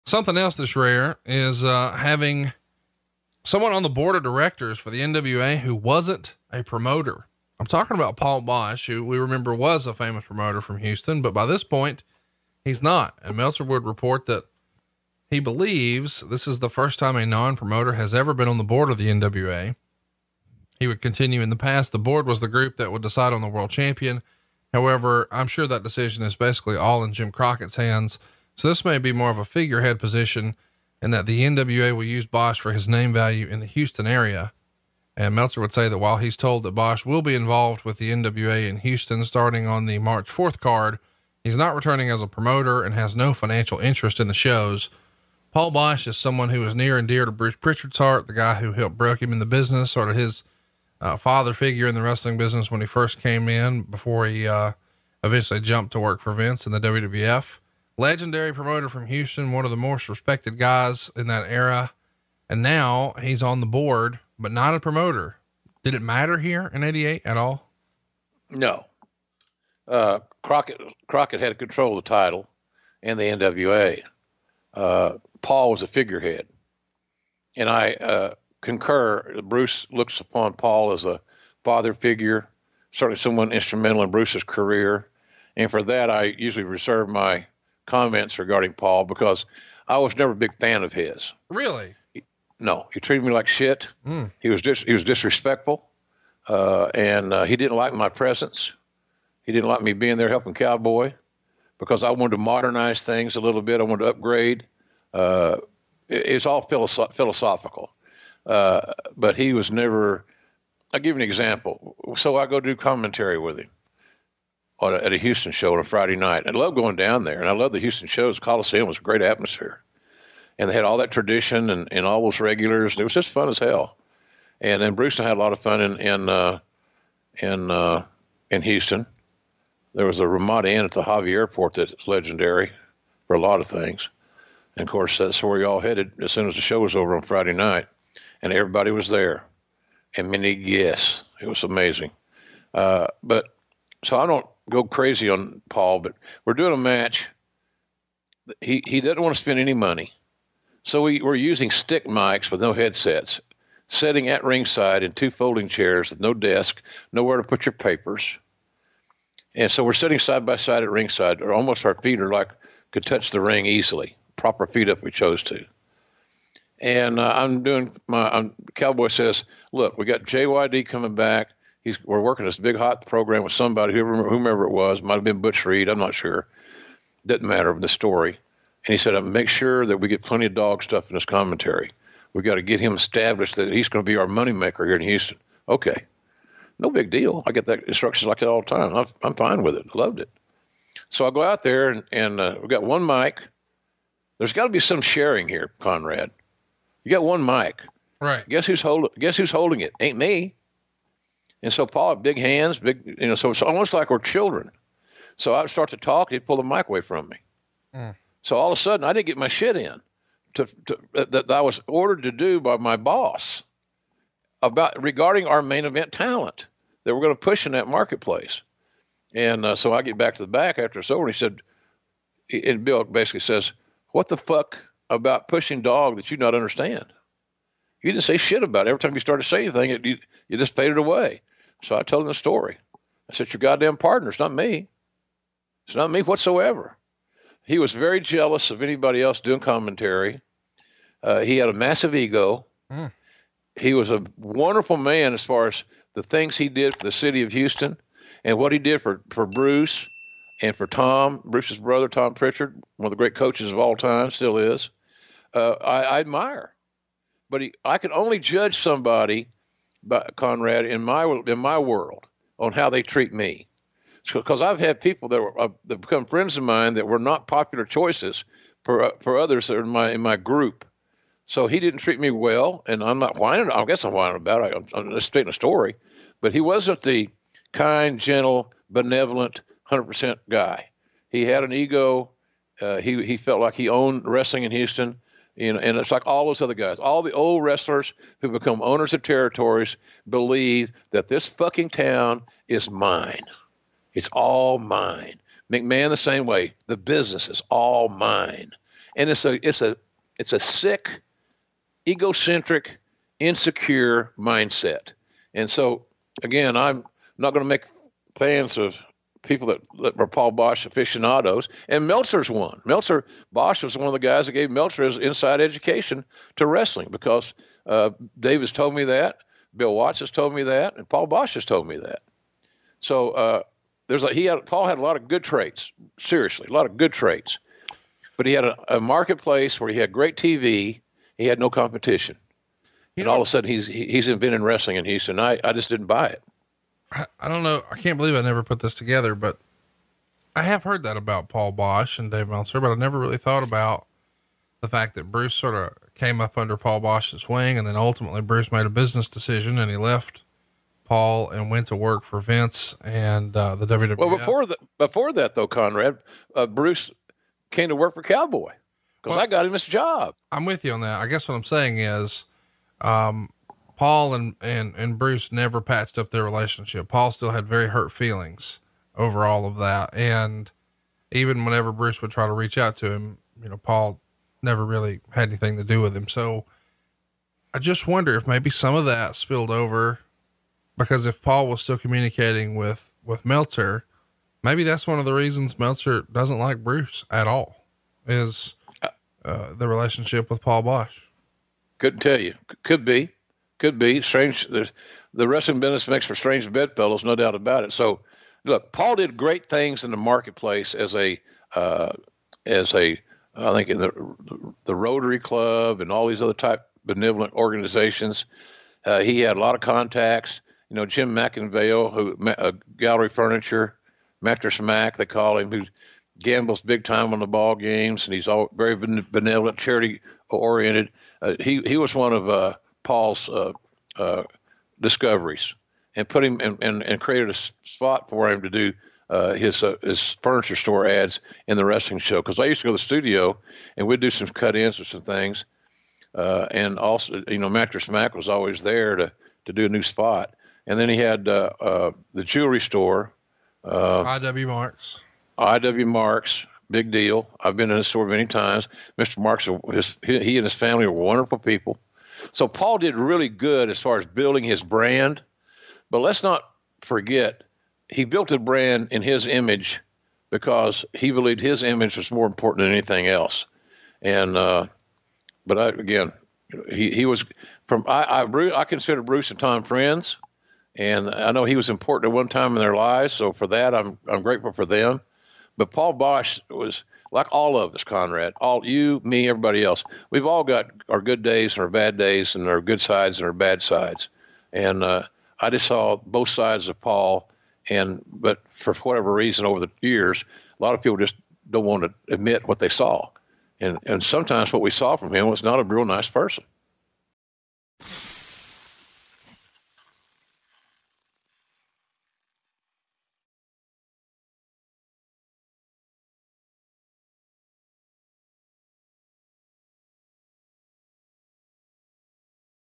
JIM ROSS: "I didn't care for Paul Boesch"
Jim Ross explains why his interactions with longtime Houston, Texas promoter, Paul Boesch was less than ideal.